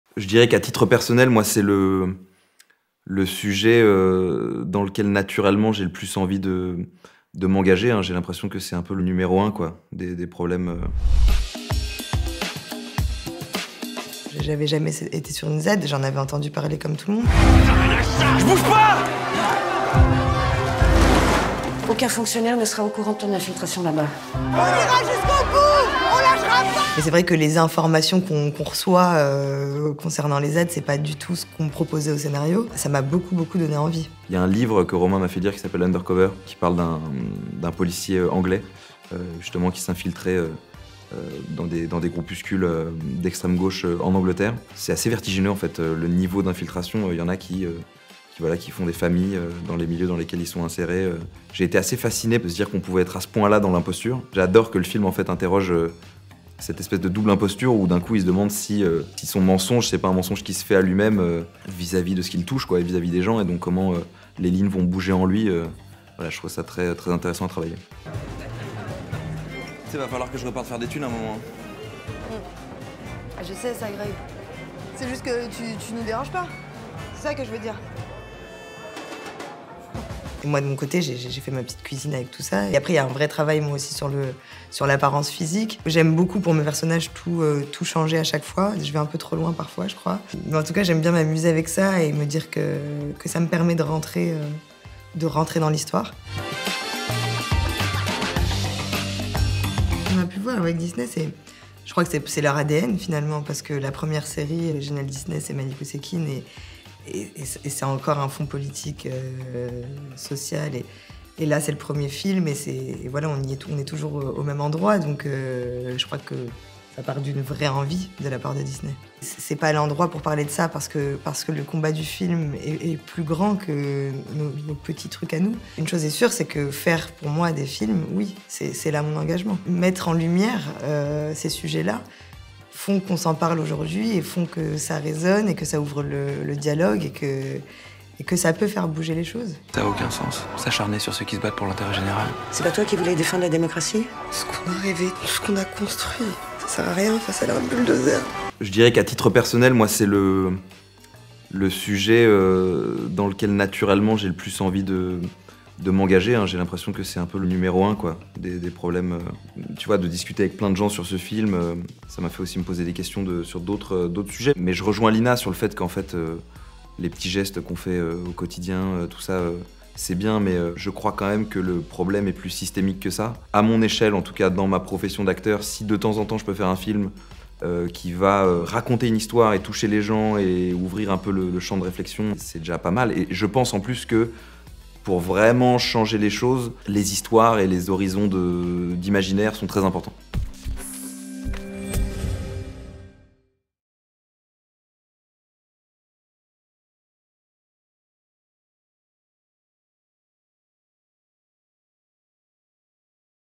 François Civil en flic infiltré et Lyna Khoudri en militante écologiste, un cocktail explosif ?
Dans Une Zone à Défendre, François Civil interprète Greg, un officier de la DGSI envoyé sous une fausse identité pour infiltrer une ZAD. Il y rencontre Myriam, incarnée par Lyna Khoudry, une militante écologiste. Nous avons rencontré les deux acteurs à l'occasion de la sortie du film sur Disney+ pour savoir ce qui les avaient attiré dans ce projet et s'il s'agissait d'un film engagé.